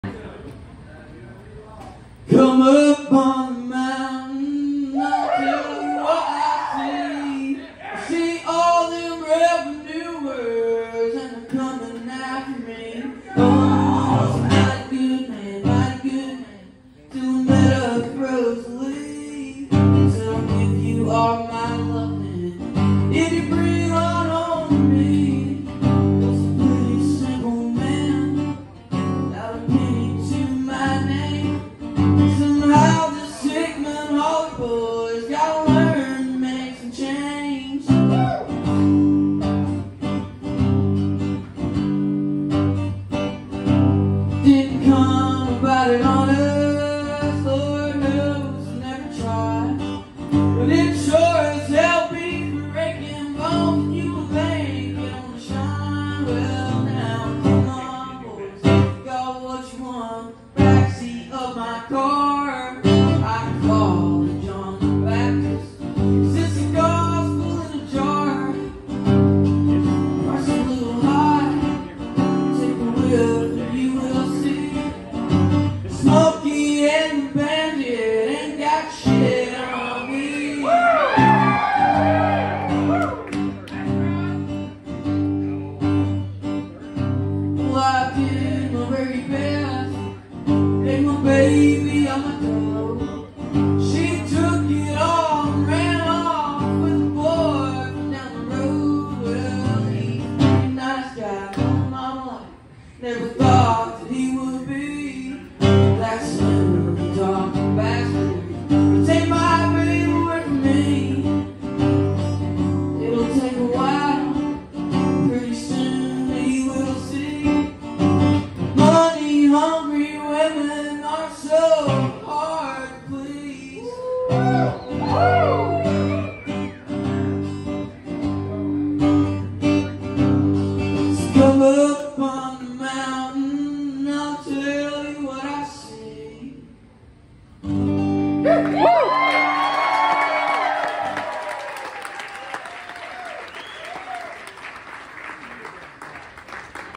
Logan Halstead - Unreleased Tyler Childers Cover (Pineville, KY 12/18/21)
Logan Halstead
Cover of Unreleased Tyler Childers Song
Jingle Bells at the Bell
Bell Theater
Pineville, KY
12/18/21